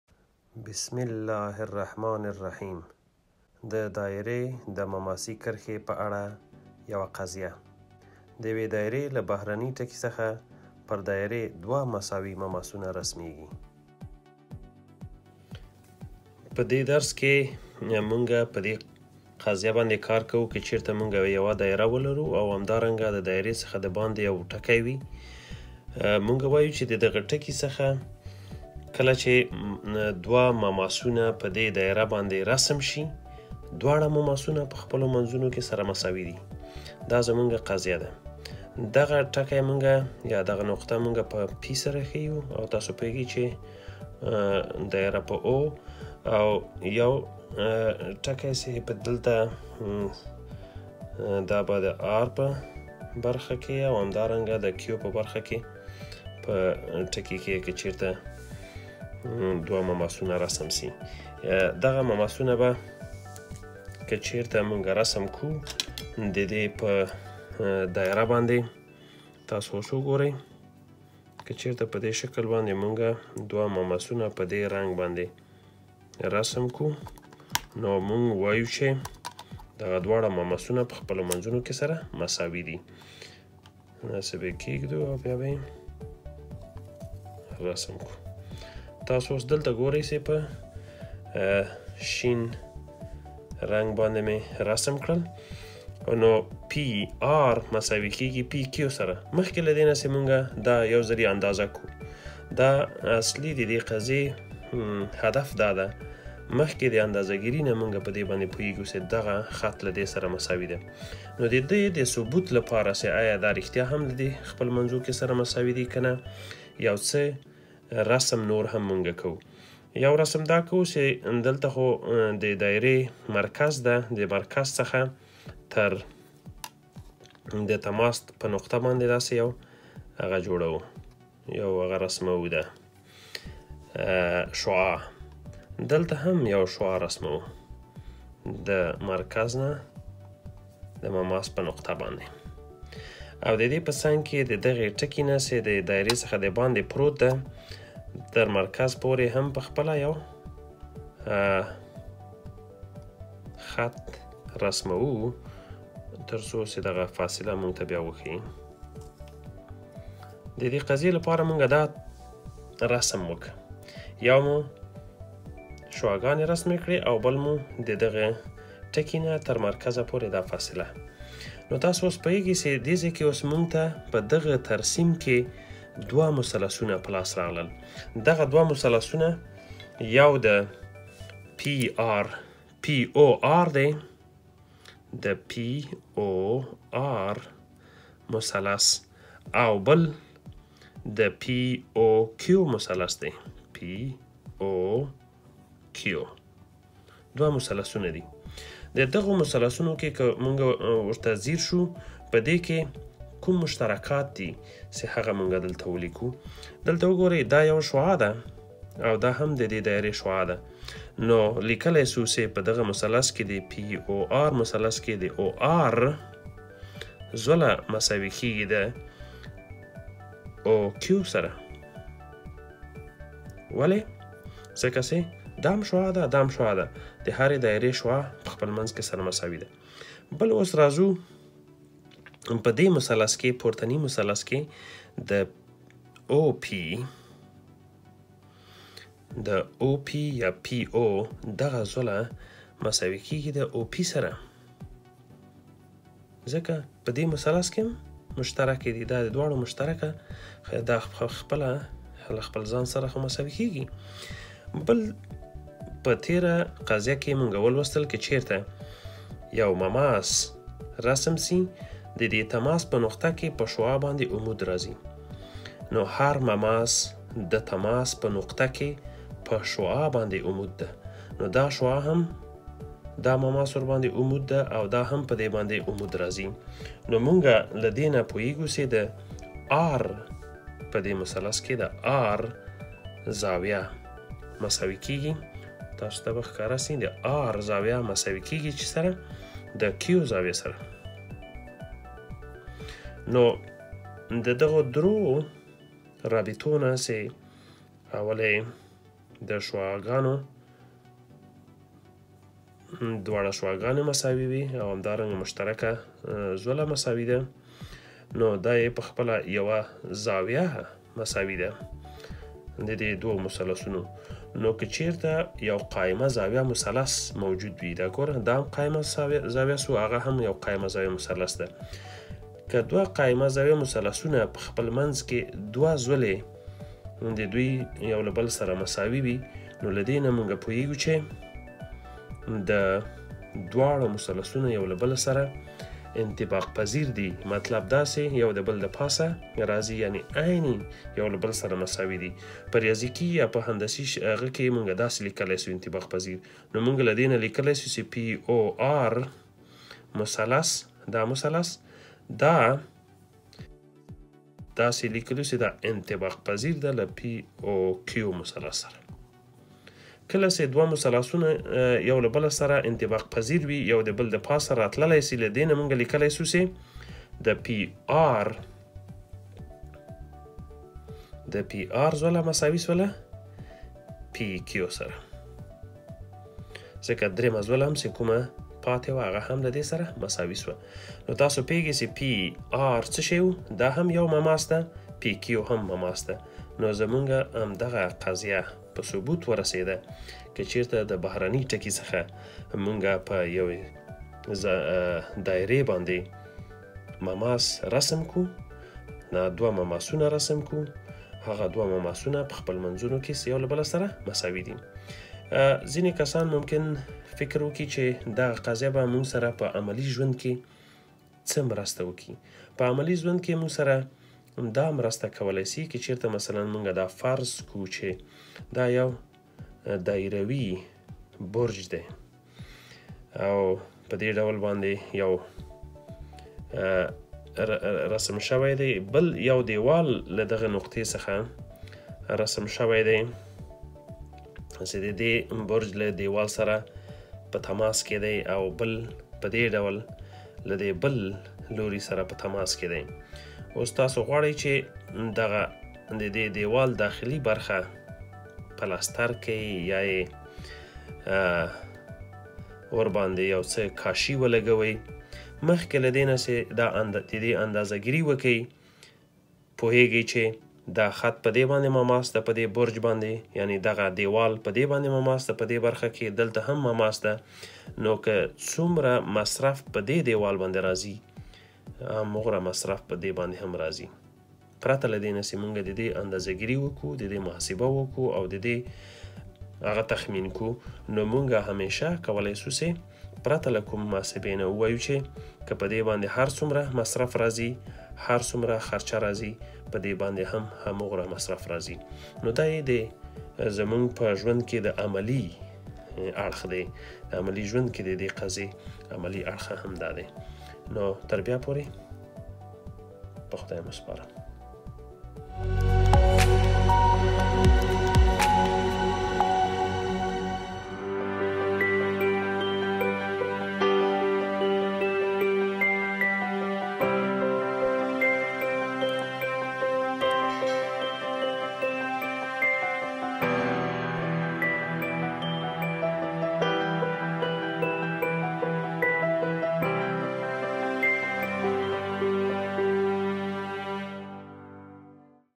د دايرې مماسي کرښه
په دې درس کې به شاګردان زده کړي چې څنګه یوه مماسي کرښه له دايرې سره اړیکې لري.

په دې درس کې دا قضیه هم په ثبوت رسېدلې ده چې له یوه بهرني ټکي څخه پر دايره باندې دوه مماسونه رسمېږي چې دوی دواړه له یو بل سره مساوي وي.
په پای کې د دغې قضیې عملي اړخ ته هم اشاره شوې چې په ورځني ژوند کې موږ څنګه له دې قضیې څخه ګټه اخیستلای شو؟